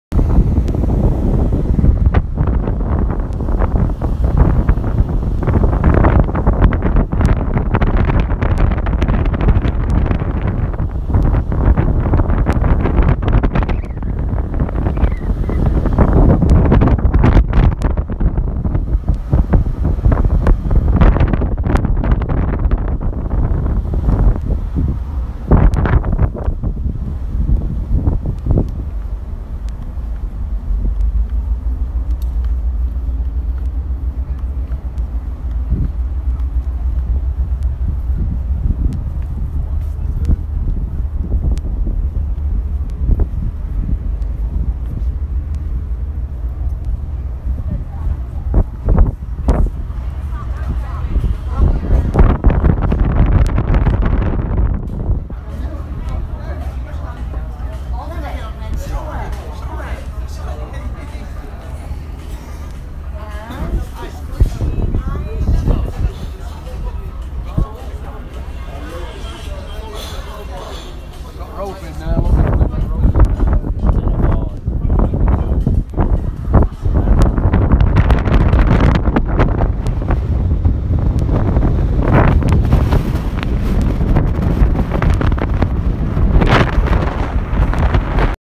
On the Costa Luminosa getting fuel in Tenerife
I can’t even begin to explain this clip other than it’s been 24 hours since Costa informed their guests that they were on another virus ship. One passenger died on Saturday in the Cayman Islands after debarking and another couple has tested positive after being found to have pneumonia.